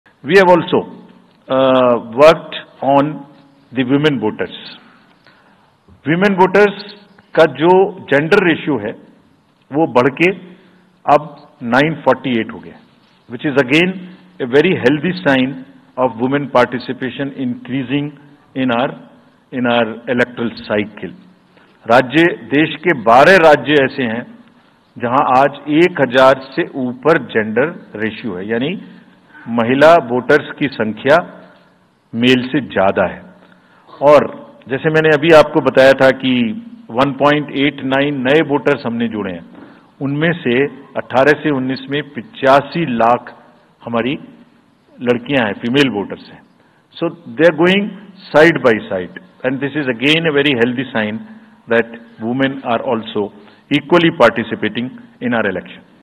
CEC Rajiv Kumar Praises Increase In Number Of Women Voters
Chief Election Commissioner Rajiv Kumar assures the nation of a vibrant and democratic atmosphere as the Lok Sabha and Legislative Assembly elections approach. His pledge comes amidst preparations for elections in several states, including Andhra Pradesh, Odisha, Arunachal Pradesh, Sikkim, and Jammu and Kashmir.